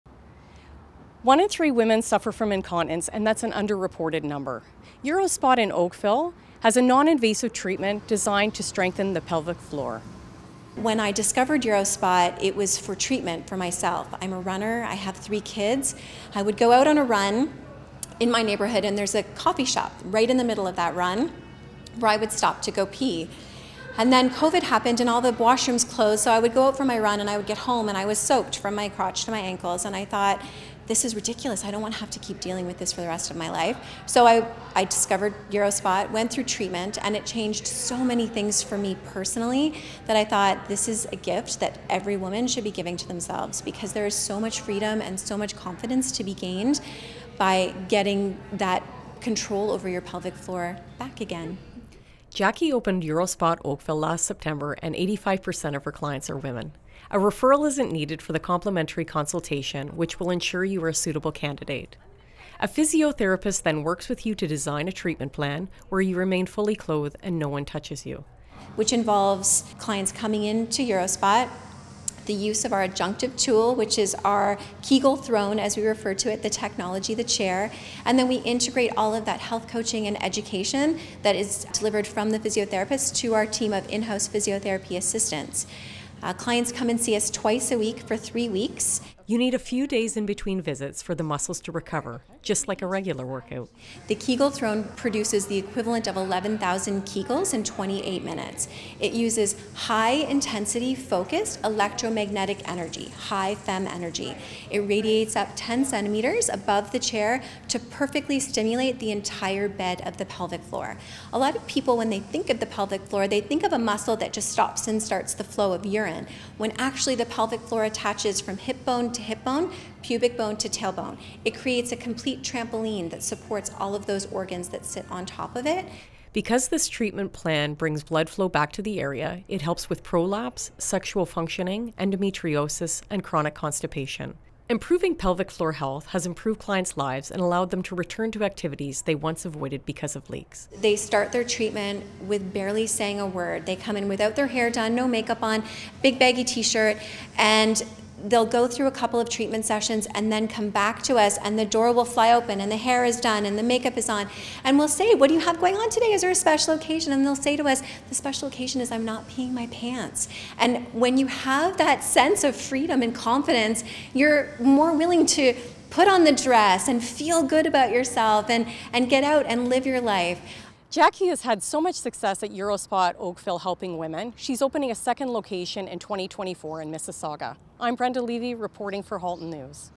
UROSPOT Oakville
UROSPOT is working to help clients live life to the fullest helping to strengthen their pelvic floor and prevent incontinence with new technology they affectionately call the "kegel throne."